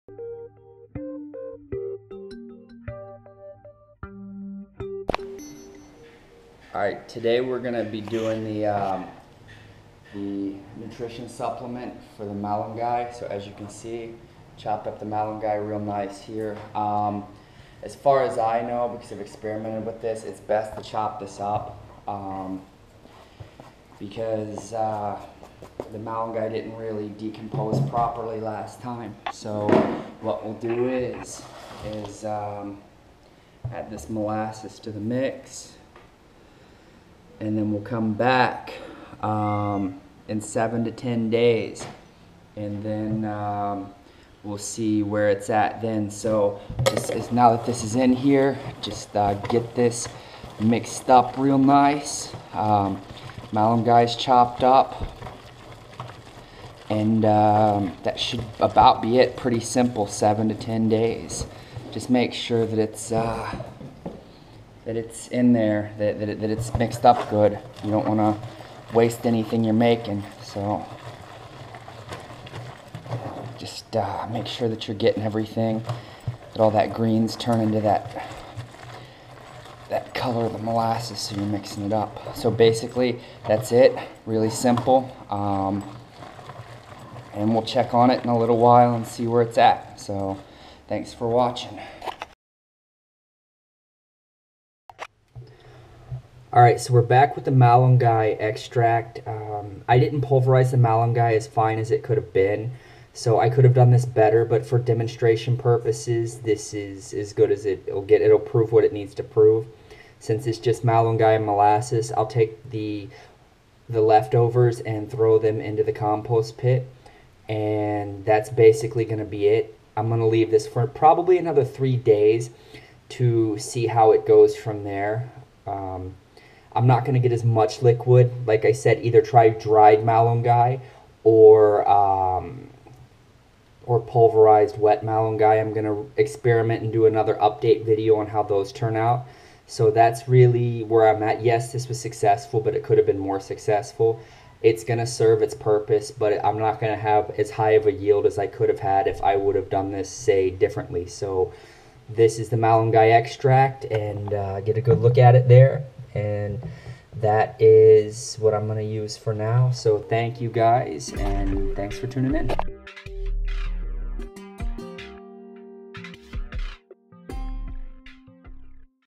How To Make Moringa (Malunggay) Extract
Moringa (Malunggay) Extract

This is a great ... For your garden/soil/plants.

 1 kilo of finely shredded moringa leaves

1 liter molasses

7 days stored dark area after thorough mixing

Basically this is another great nutritional supplementation for plants and animals.

Same ratio as before 2 tsp to 1 liter of water.